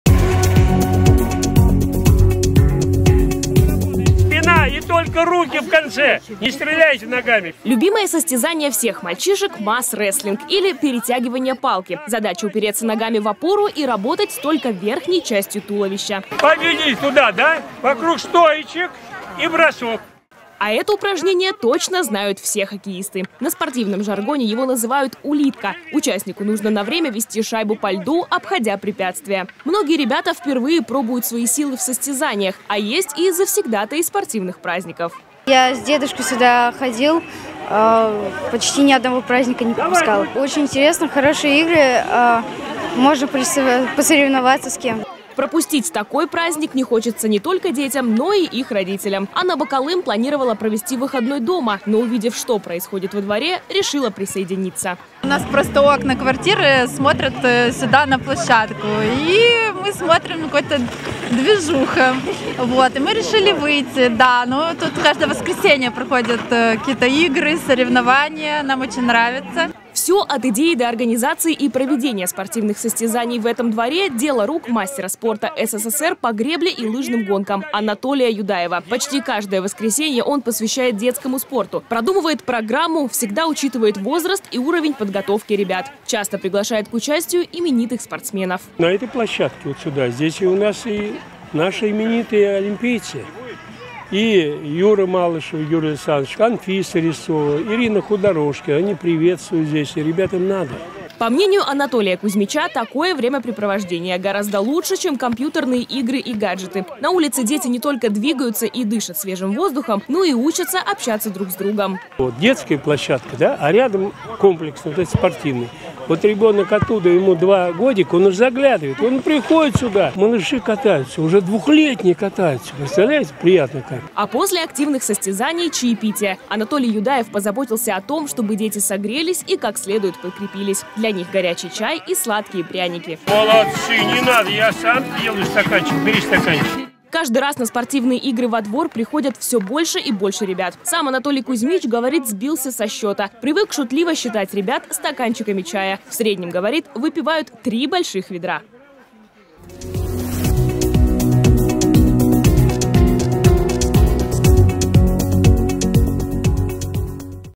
Каждые выходные мастер спорта СССР устраивает соревнования для детей. 8.02.2022
Любимое состязание всех мальчишек – «мас-рестлинг» или «перетягивание палки». Задача упереться ногами в опору и работать только верхней частью туловища.